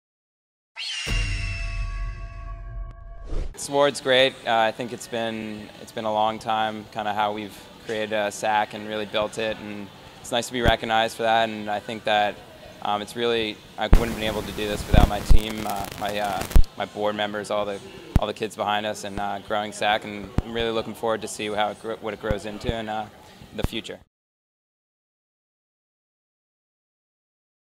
Golden Eagles: SAAC Award (May 5, 2019)
Aidan Durney from Men's Track and Cross Country discusses what this award means to him.